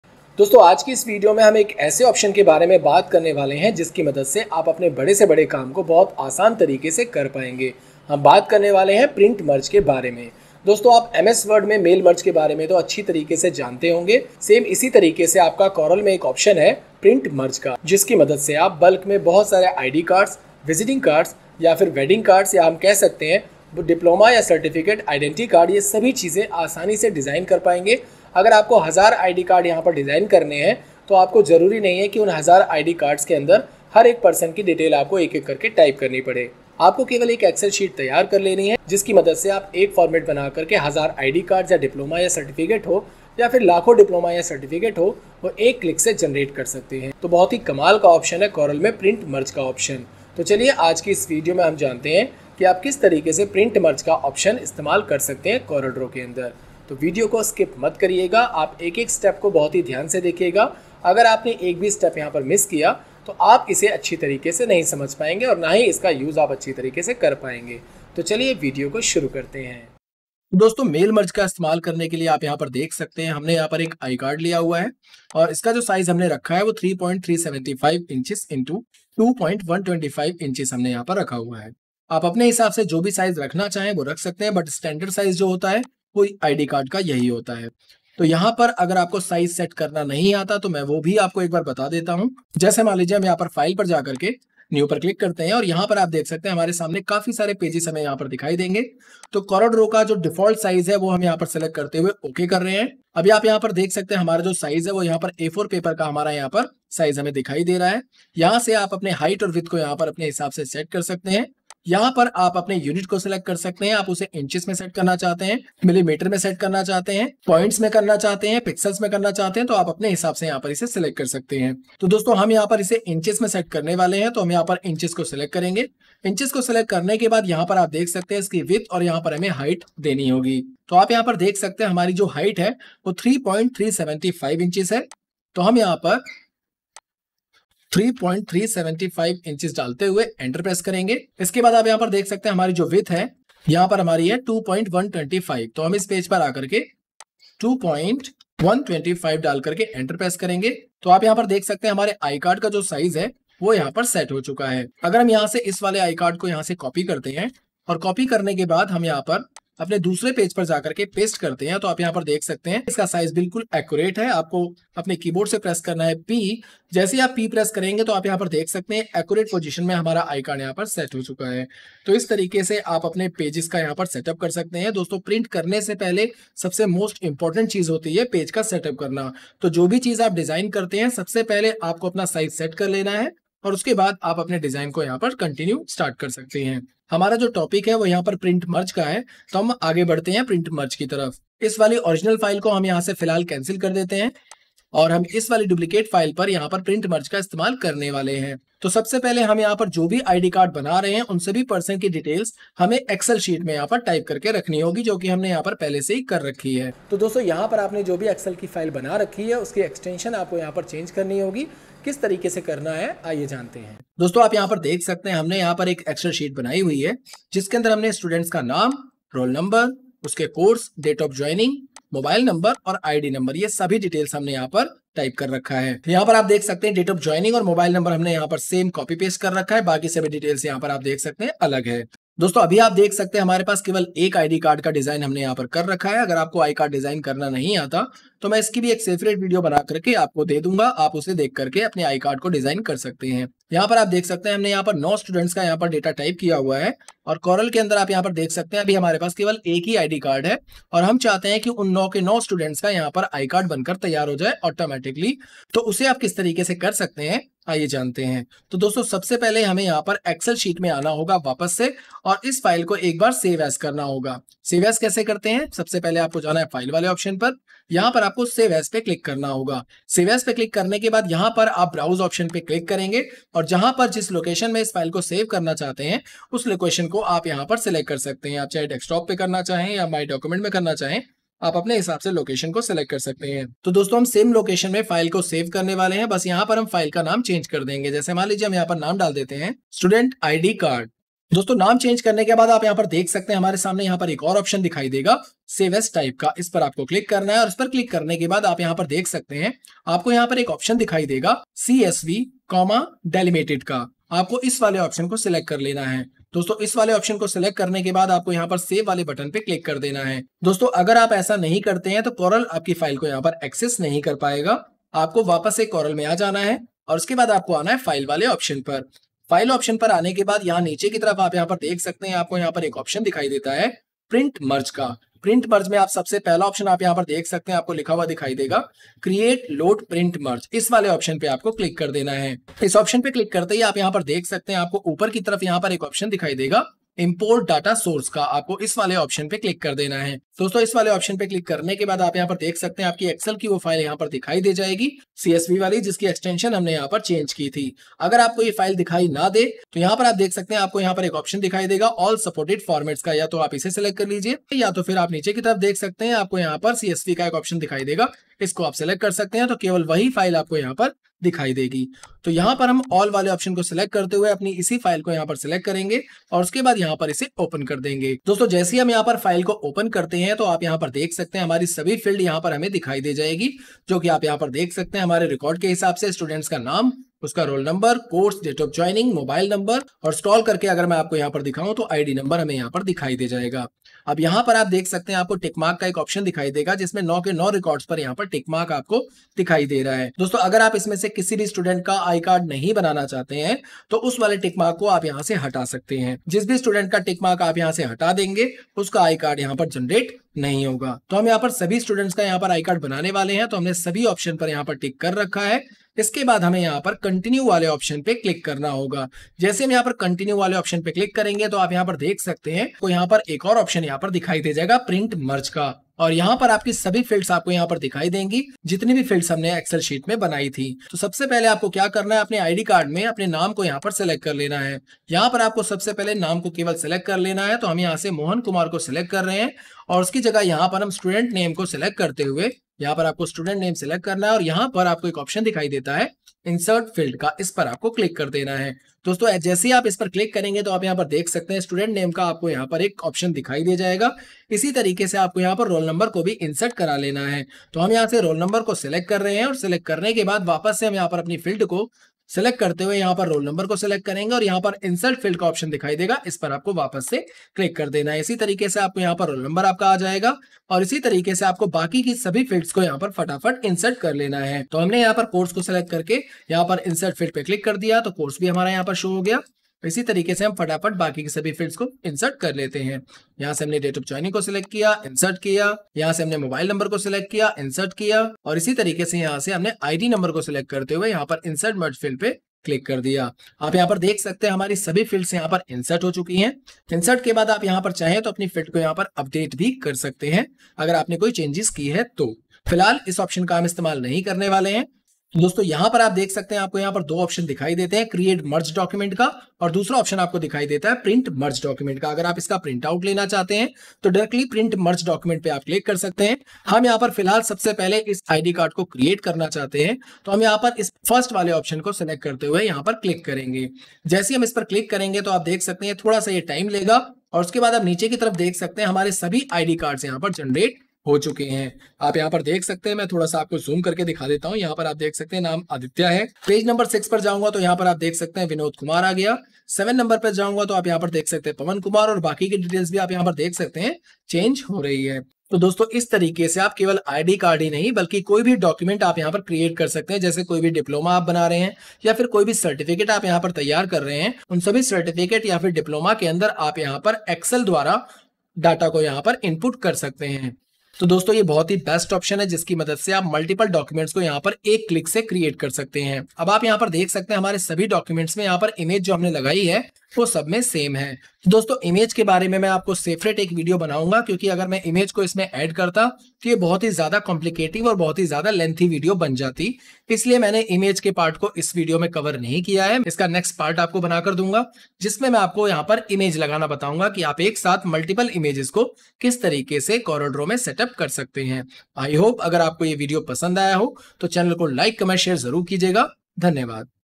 Print Merge in CorelDraw 2024 | Student Identity Card with Print Merge option in Corel Draw 2024
In this tutorial I will show you how to import and work with variable text/data using print merge in combination with Excel and CorelDRAW. In my opinion the best and fastest way.

Time Stamp:
00:00 Introduction
01:21 How to page size setup in corel draw 2024
03:18 How to use print merge in corel draw 2024
04:24 How to make excel file to .CSV File
05:25 How to use Create/Load Print Merge in Corel Draw 2024
05:40 How to Show CSV File in Corel Draw 2024
06:50 How to insert Feld From Print Merge in Corel Draw 2024
08:15 How to use Create Merge Document in Corel Draw 2024